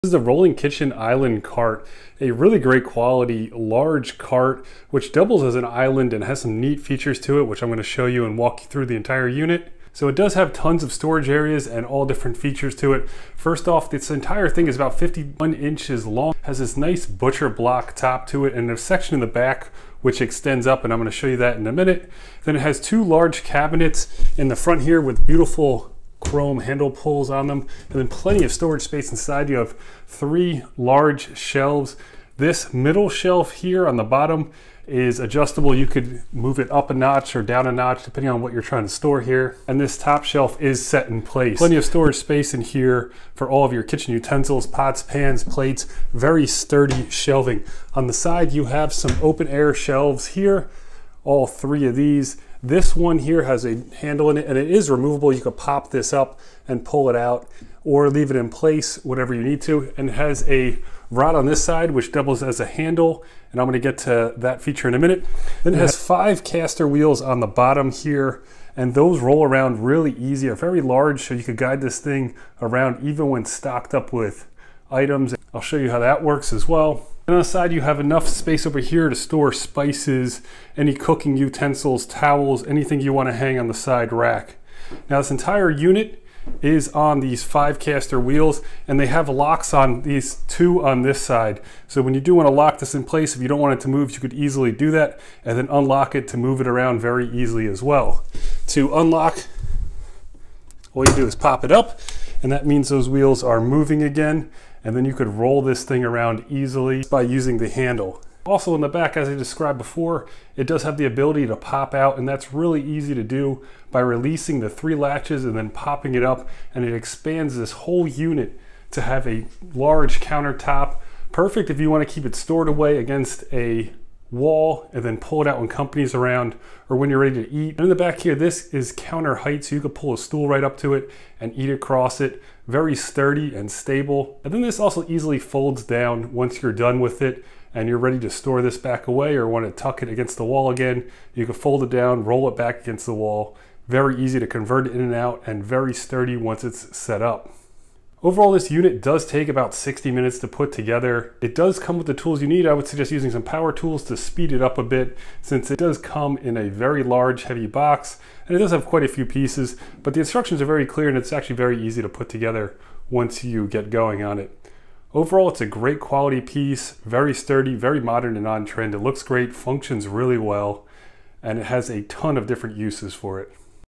Details, Demo, & Full Review of the Gizoon Rolling Kitchen Island
Gizoon Rolling Kitchen Island with Drop Leaf, 51" Width Wood Countertop, Kitchen Carts on Wheels with Large Storage Cabinet, Adjustable Shelf, Spice Towel Rack, Removable Tray, White